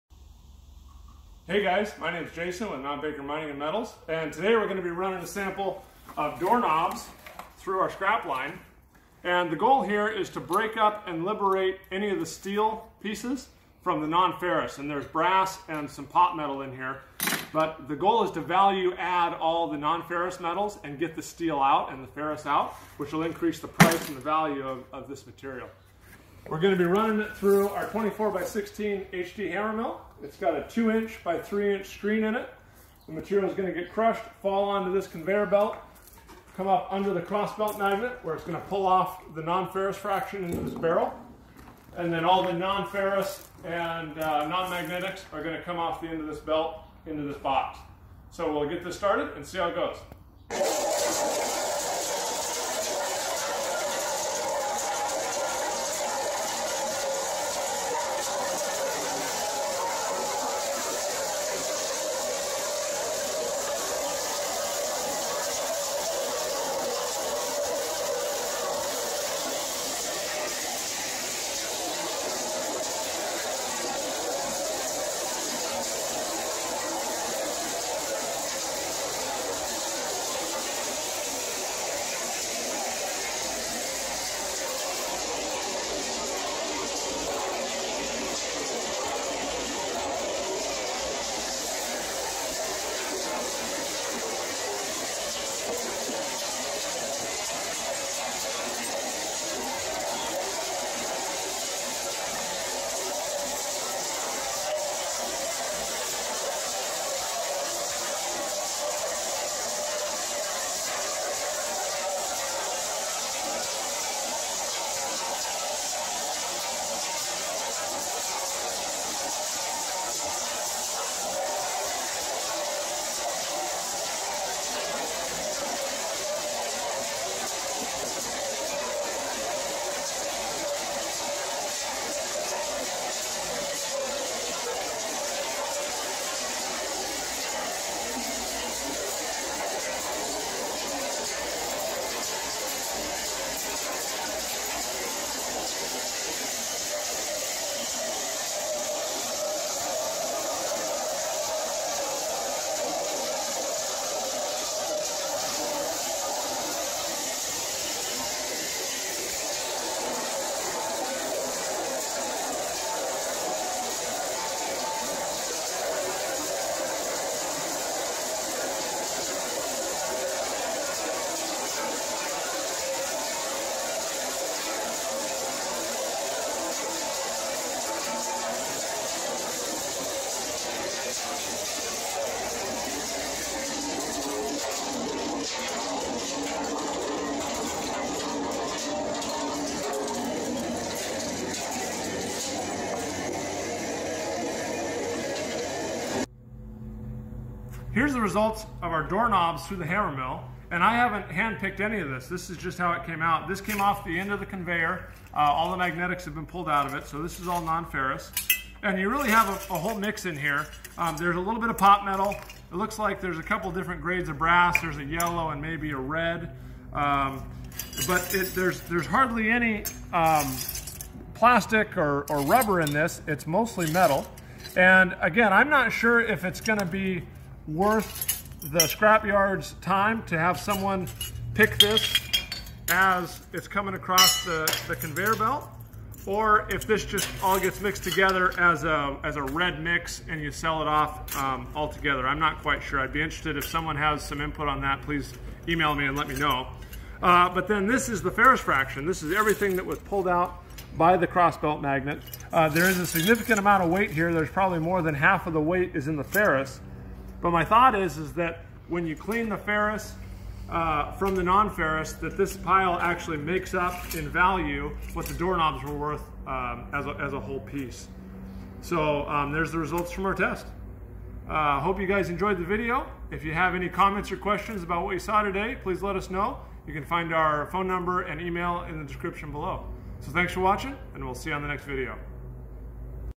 Hammer Mill Crushing Door Knobs to Recover Clean Brass
Running a sample for a customer of door knobs to recover the clean brass from the steel.  This customer has a whole warehouse full of door knobs and he wants to recover the value in the brass.  The sample ran great with a good separation of the ferrous and the non ferrous.

Smelting Supplies:
Crucibles: #4 10# fire clay

Underground Mining Equipment:
Feather and wedge set